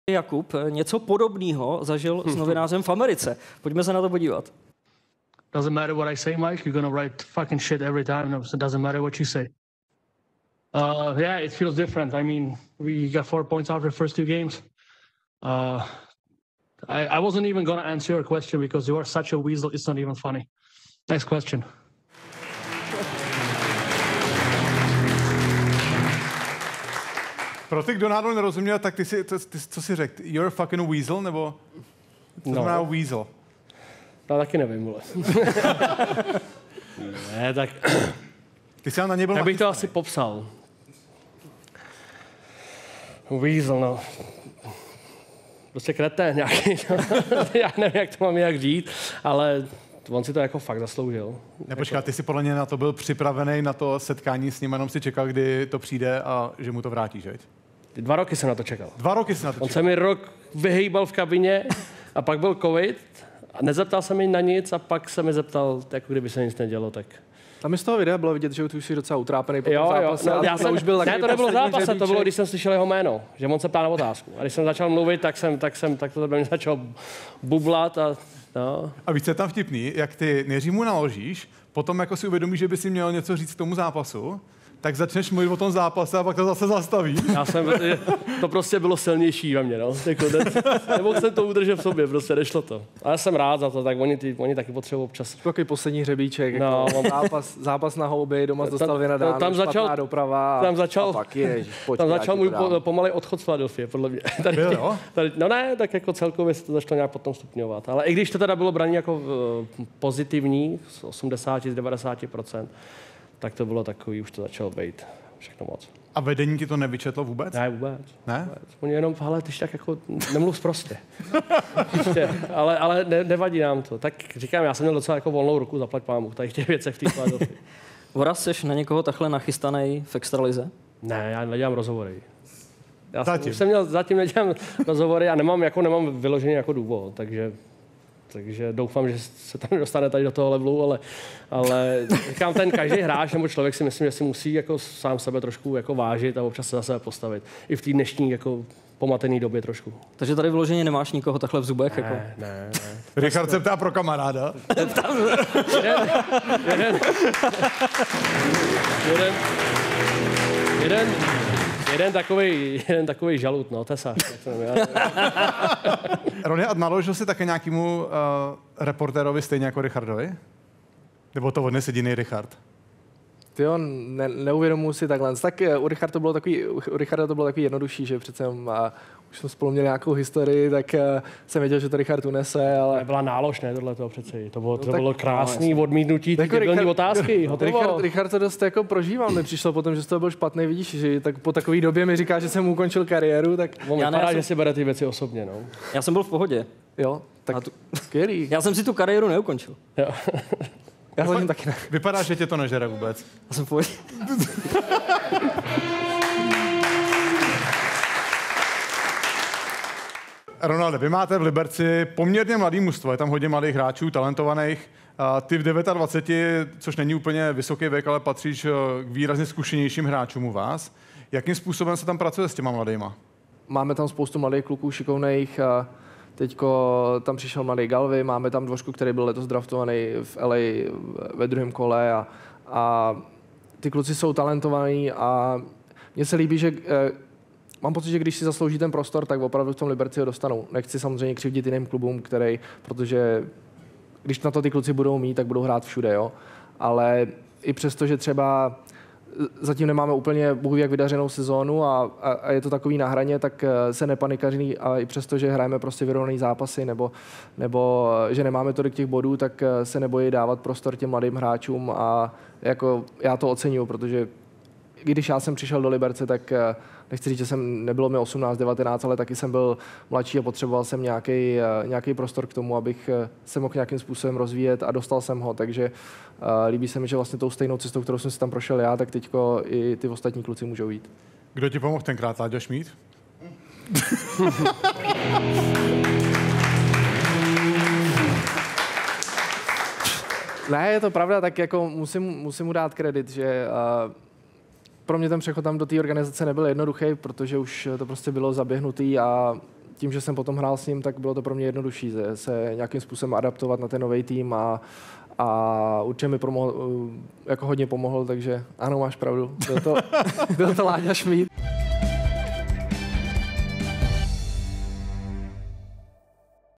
Voráček o uzemnění novináře: Fakt si to zasloužil, čekal jsem na to dva roky
Pamatujete si, jak Jakub Voráček při působení ve Philadelphii pořádně uzemnil novináře? "On si to fakt zasloužil. Dva roky jsem na to čekal, rok se mi vyhýbal v kabině, pak byl covid. A pak se mě zeptal, jako by se nic nedělo," zavzpomínal s úsměvem. A co na to říkali ve vedení Flyers?